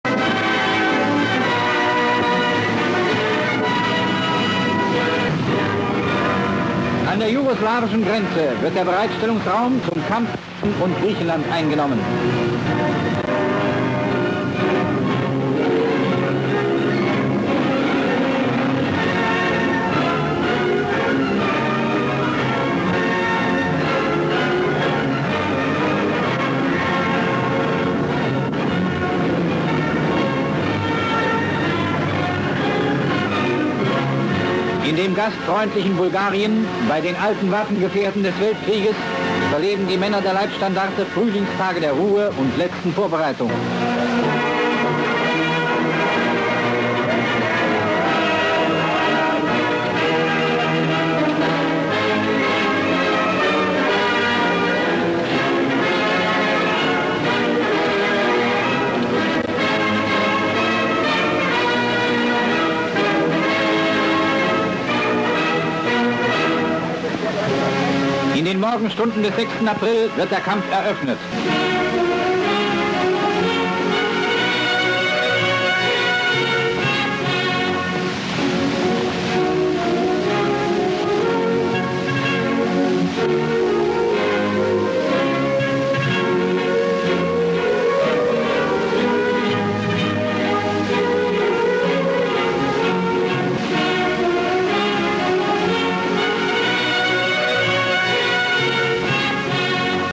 LSSAH Villagers
The LSSAH meets local villagers on the way to Yugoslavia and Bulgaria.